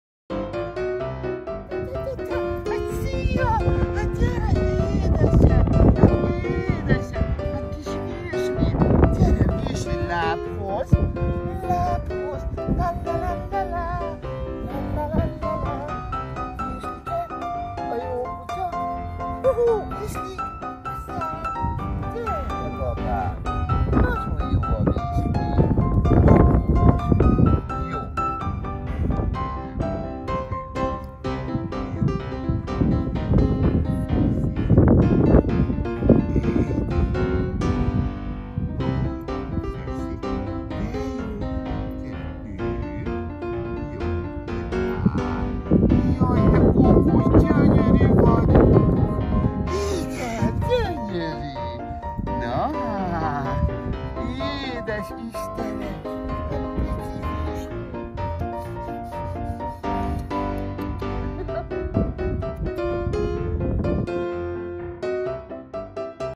Virsli a csepp corgi első bölcsis foglalkozása
A 10 hetes csepp kereszt gyerköcöm első bölcsis foglalkozásának pillanatai :-) Nyitott, érdeklődő, kiegyensúlyozott, falatkákkal nagyon jól motiválható :-) Öröm boldogság volt az első közös óránk :-)